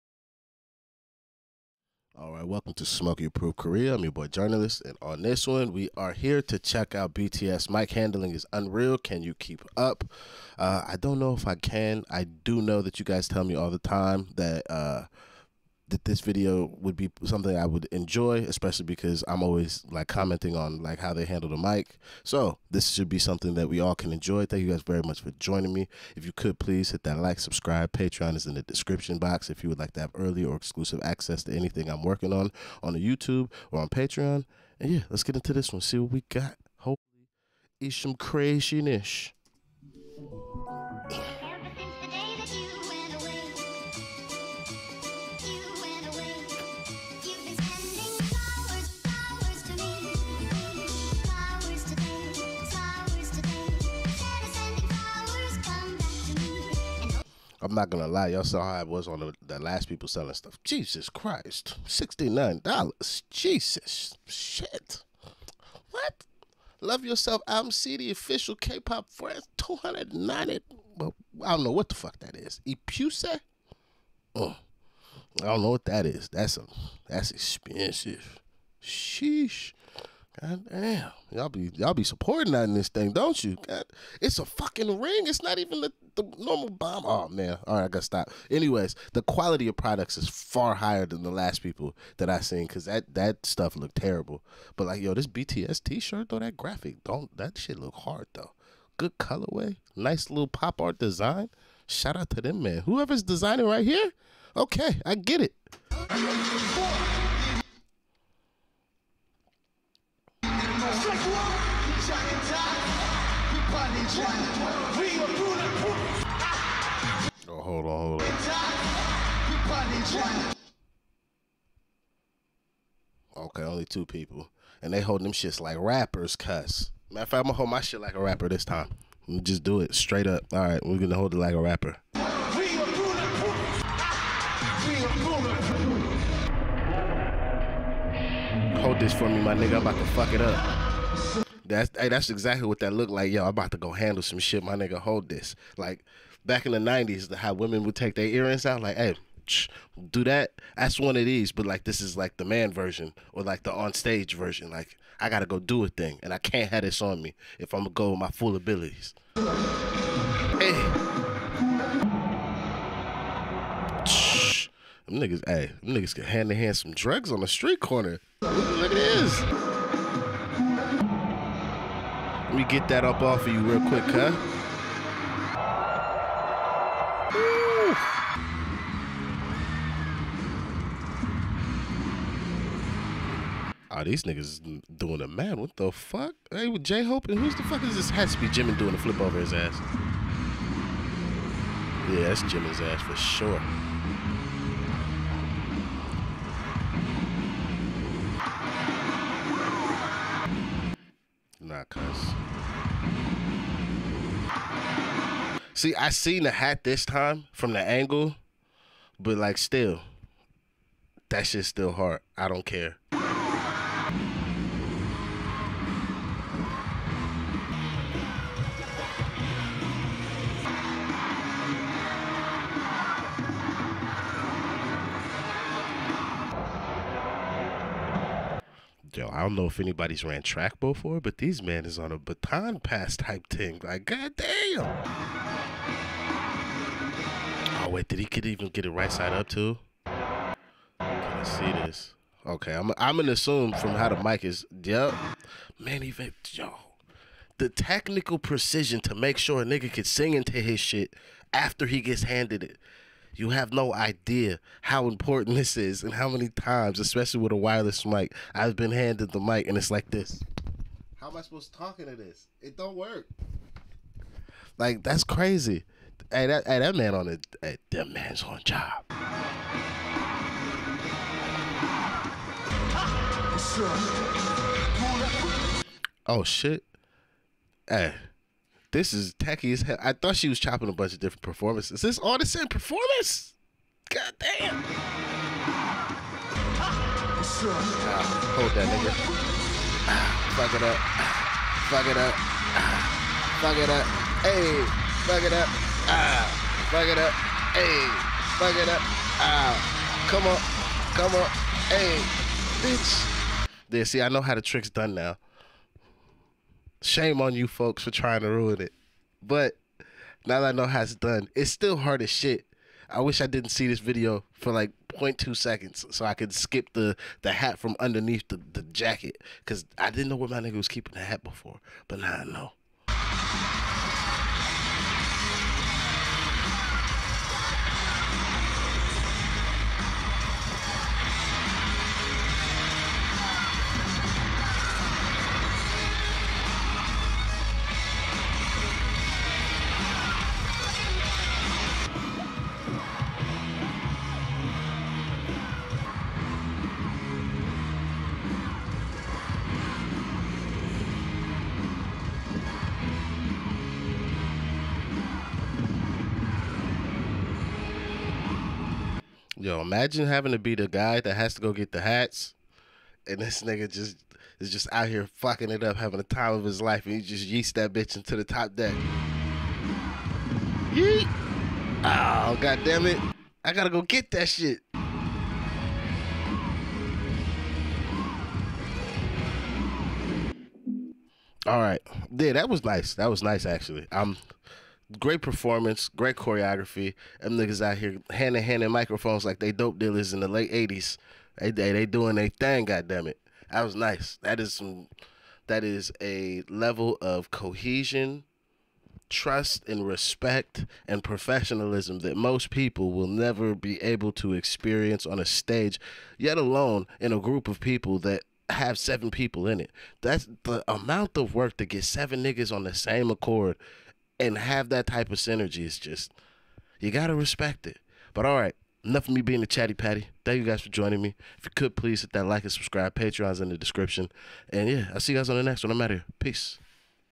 BTS's mic handling is unreal... Can you keep up?
EP83: Smokey Approved S.Korea- American Rapper First Time - BTS's mic handling is unreal... Can you keep up?